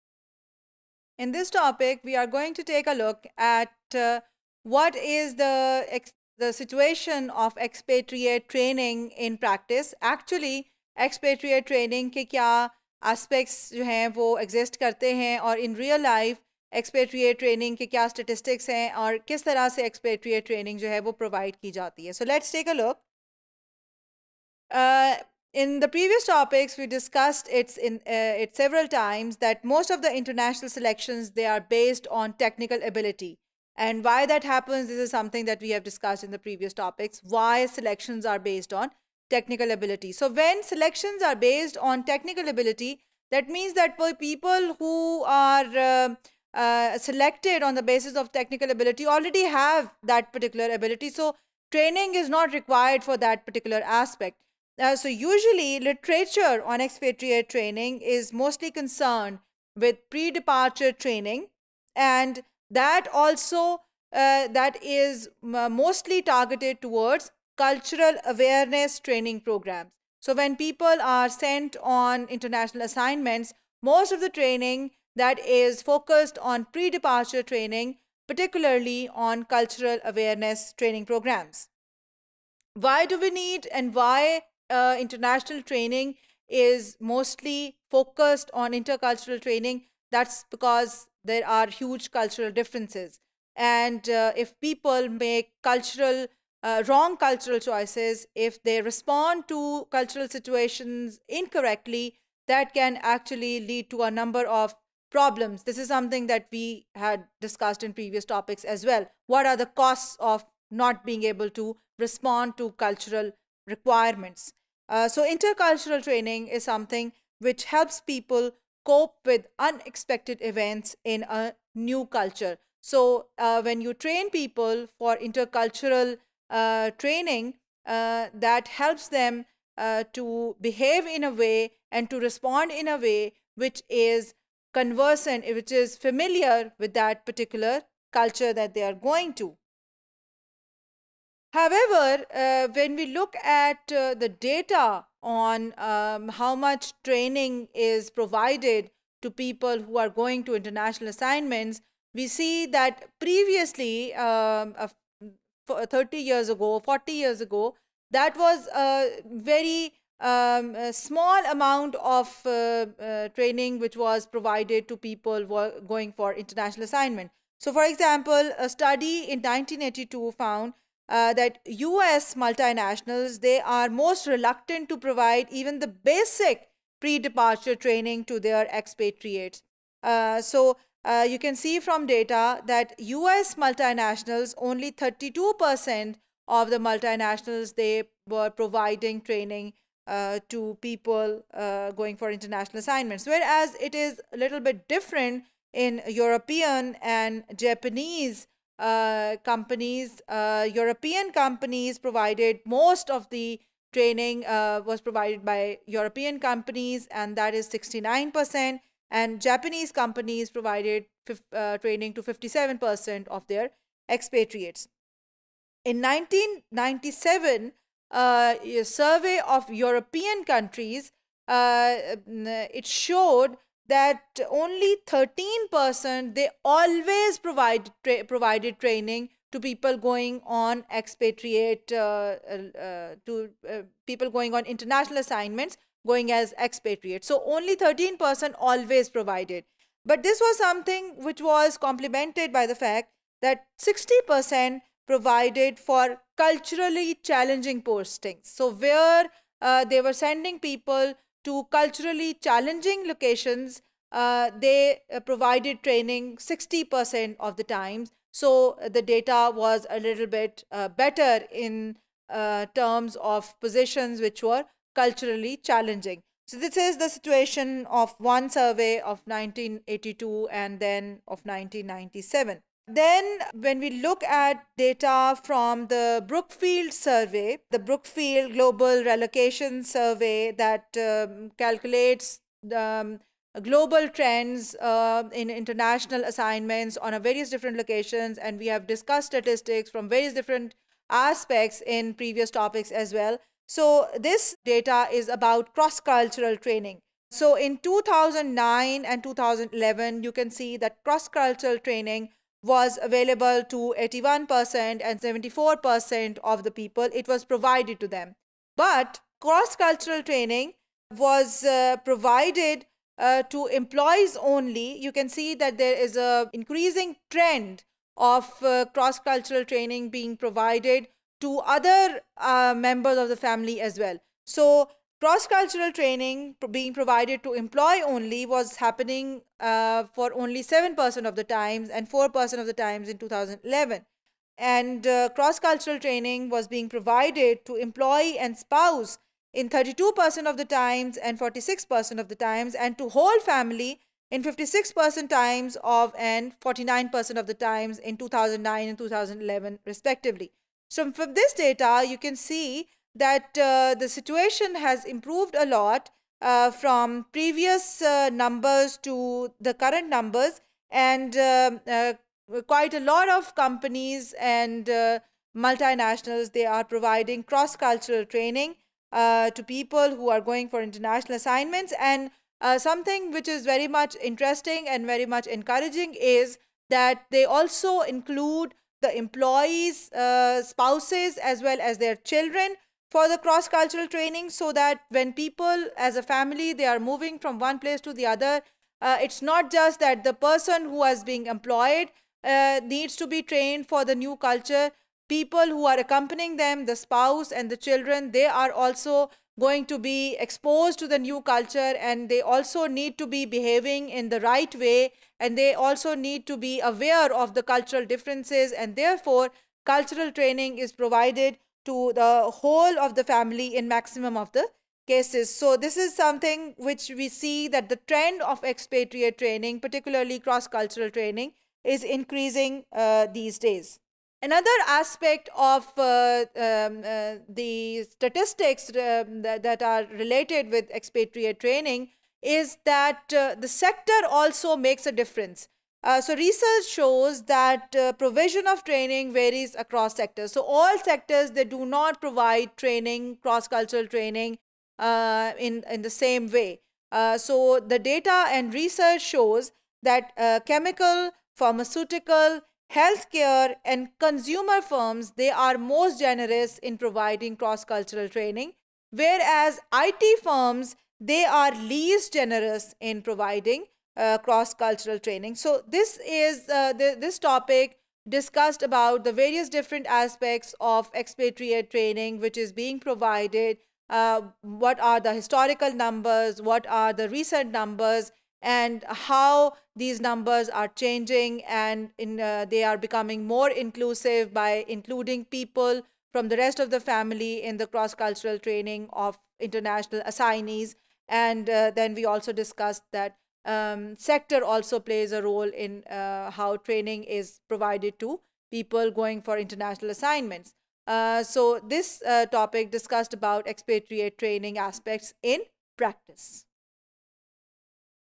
Expatriate Training in Practice | International Human Resource Management | HRM630_Topic085
Topic085 - Expatriate Training in Practice,
By Dr. Sania Zahra